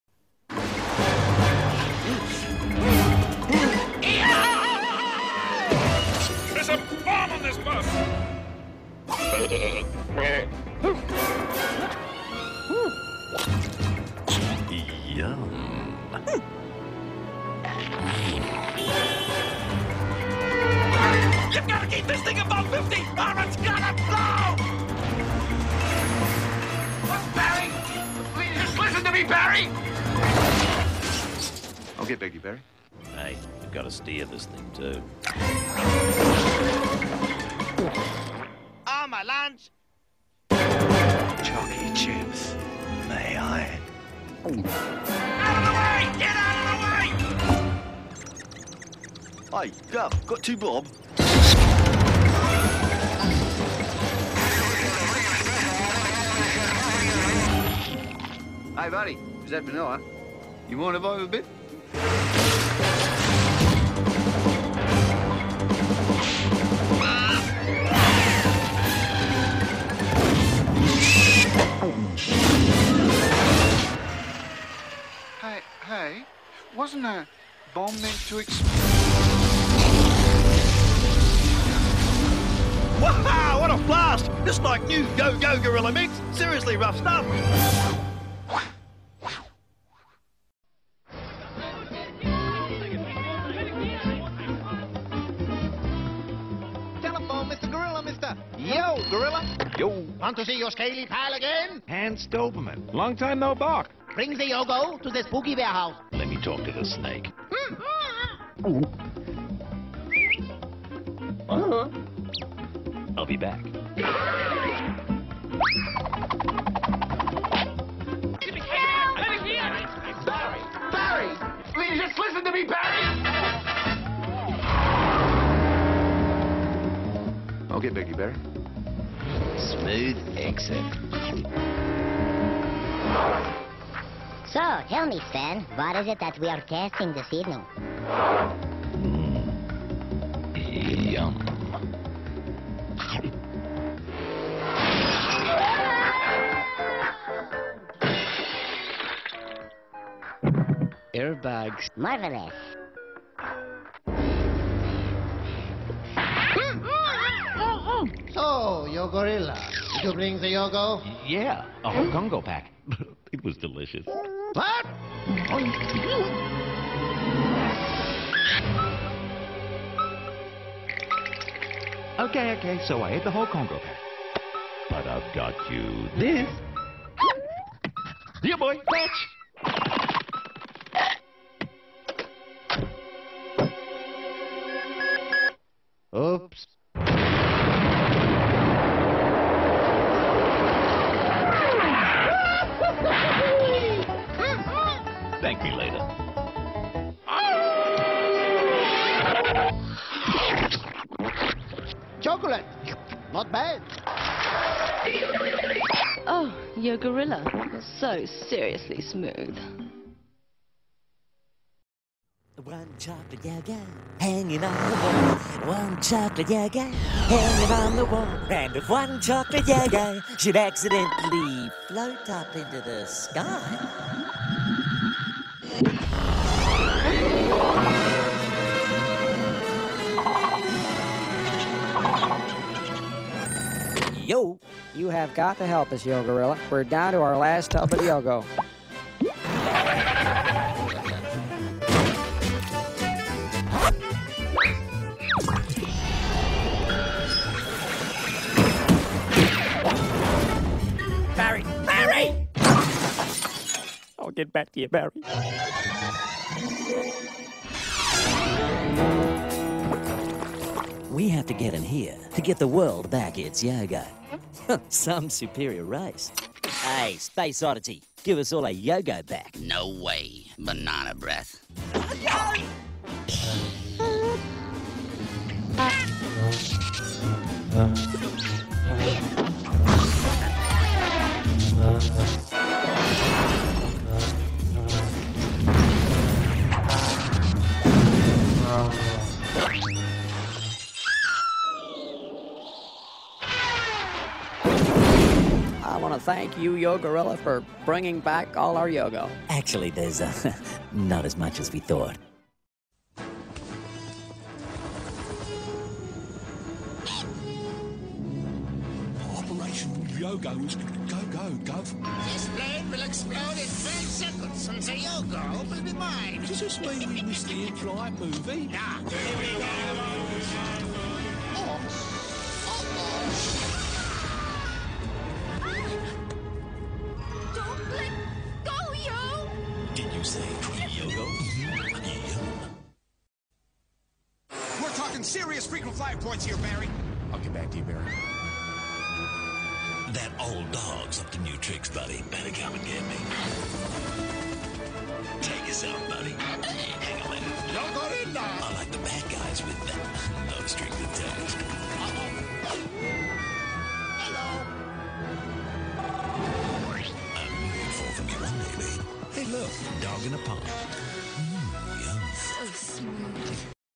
Yogo Commercials Compilation
Yogo Commercials Compilation TV Ad Collection
Yogo Speed Bus - YoGo Gorilla Mix - Seriously Rough Stuff
Planet Hollywood Yogo - YoGo Congo Pack - Seriously Smooth Stuff
Alien UFO
Cliff Hanger